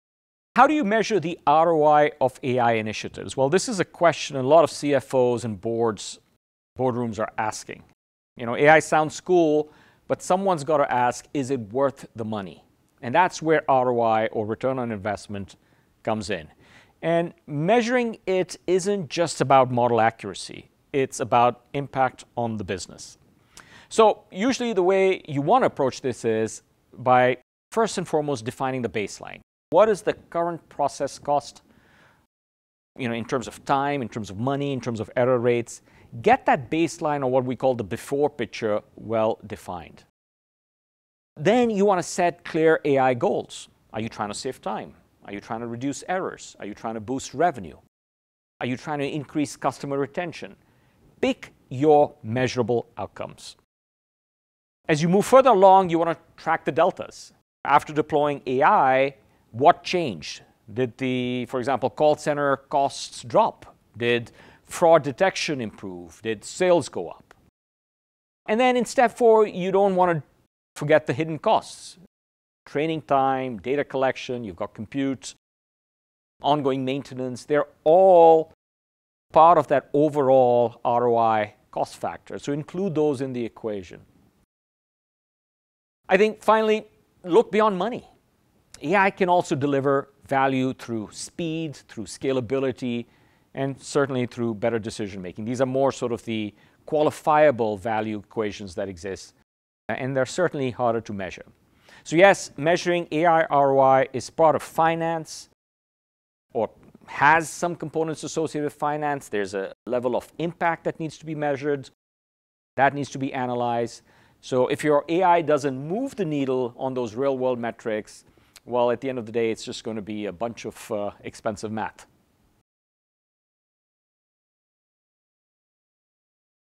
100 in 100 AI Series: Episode #28
AI should deliver real value. This 100 in 100 episode shows you how to measure return on investment—not just with accuracy, but by tracking cost, efficiency, revenue, and impact on business outcomes. Learn the steps to set baselines, define goals, track improvements, and assess the true worth of your AI projects.